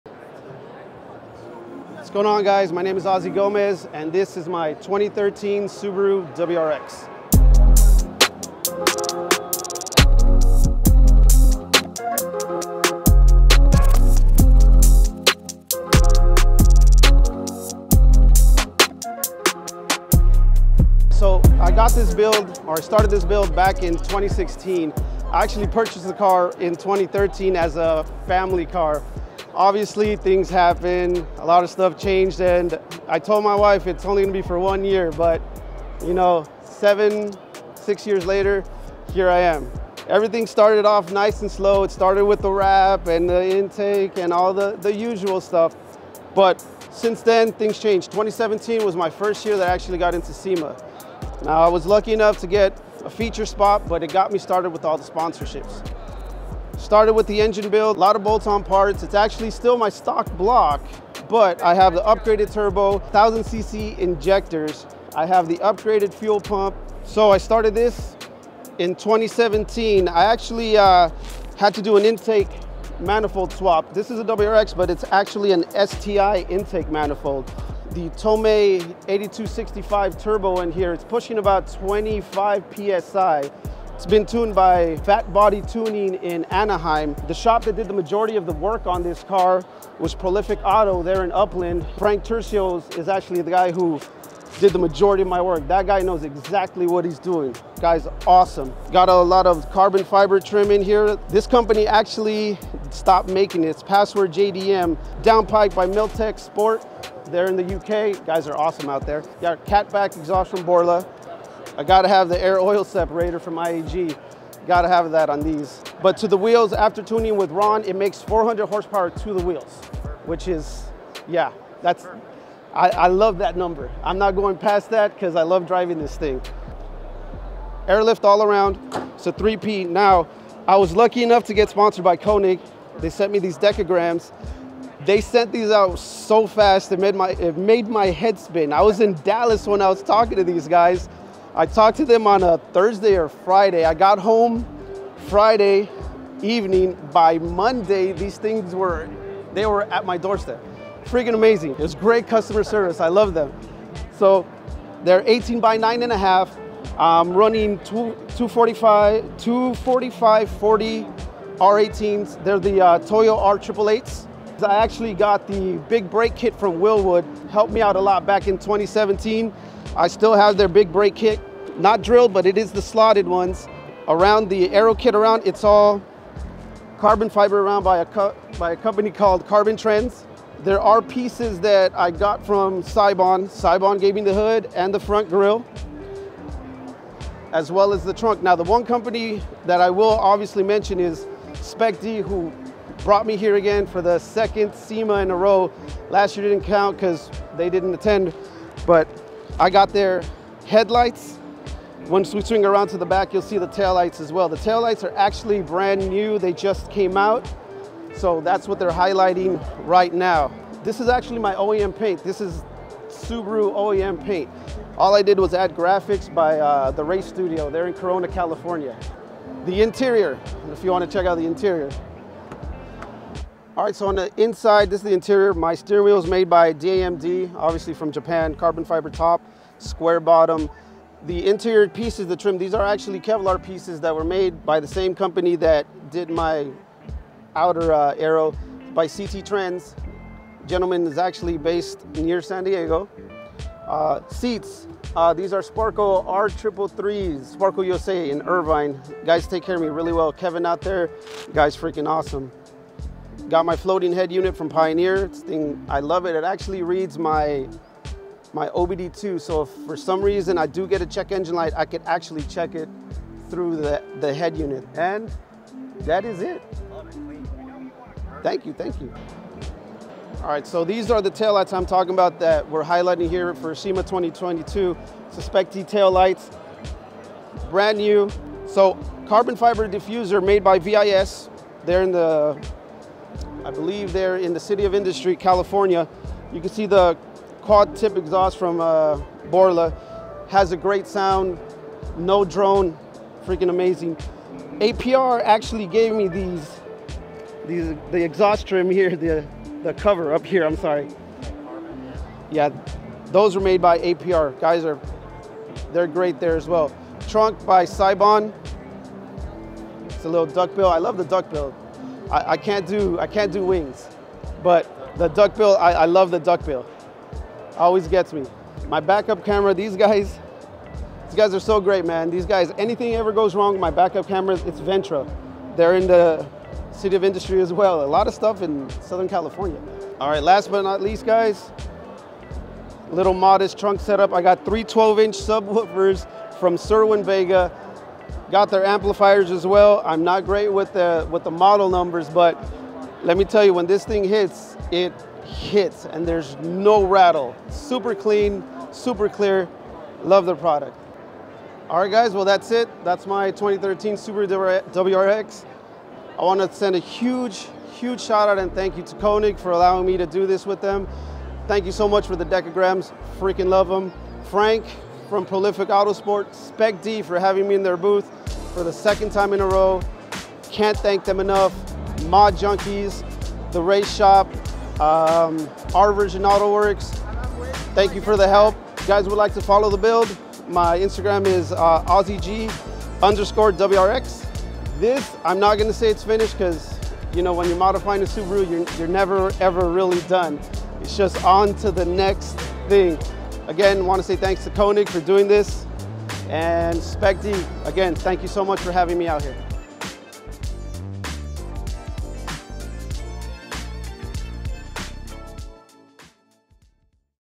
400WHP BAGGED WRX Carbon Fiber Show Car Walkaround (CARNATOMY)
We've got a STREET-BUILD that's show-car worthy on this episode of CARNATOMY. We caught up with @ozzieg_wrx at the SPEC-D Tuning Booth at SEMA and took a tour around his pride and joy, his 2013 Subaru WRX. Over the year, we've been lucky enough to tour a bunch of different builds, but it's hard to compare any of them quite to this build purely because of how unique a project it ended up becoming.

Featuring a built engine pushing 400WHP, AirLift Performance 3P System, 3x12" Subwoofers, and enough carbon fiber that would make Kaz Sakamoto proud.

Thank you to Ozzie Gomez and @Specdtuninginfo for hosting us!

Including Parts Featured by:

00:28 - purchasing the car stock
00:48 - first SEMA feature
01:00 - engine bay
01:15 - STI intake manifold
01:20 - TOMEI turbocharger
01:46 - CF engine cover kit
01:57 - power specs
02:05 - airlift performance 3P
02:10 - KONIG DEKAGRAM / Wheel & Tire Setup
02:47 - WilWood Brakes big brake kit
03:00 - Seibon carbon hood
03:22 - Spec-D sequential taillights, headlights
03:45 - interior (steering wheels, carbon fiber trim, HUD, seats)
04:47 - Borla quad-tip exhaust
05:00 - more carbon fiber
05:12 - CF Seibon duckbill trunk
05:23 - plate-mount backup camera
05:33 - "little modest" trunk setup (amplifiers, subwoofers)